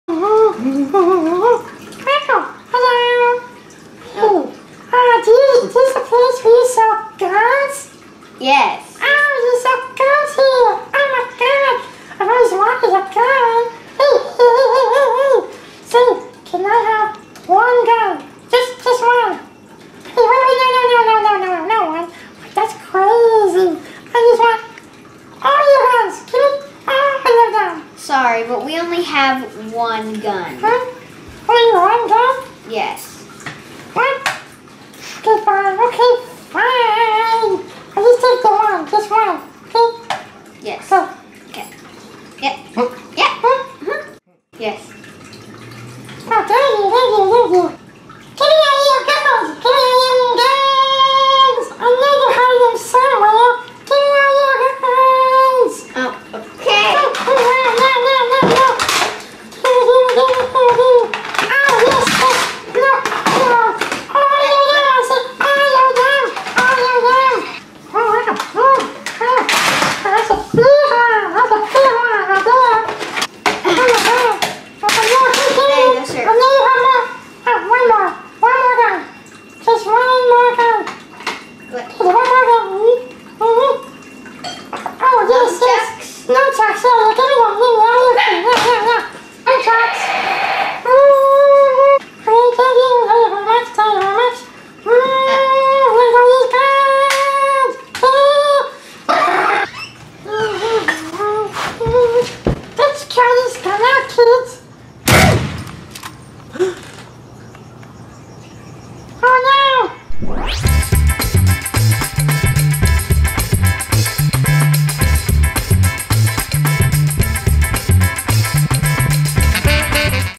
Elmo Buys a Gun! (GONE WRONG) 2017
In this video, Elmo robs a weapon store and tries to shoot his very first gun!

STARRING

Zoey Rice as the Weapon Store Owner

Zander Rice as the Voice of Elmo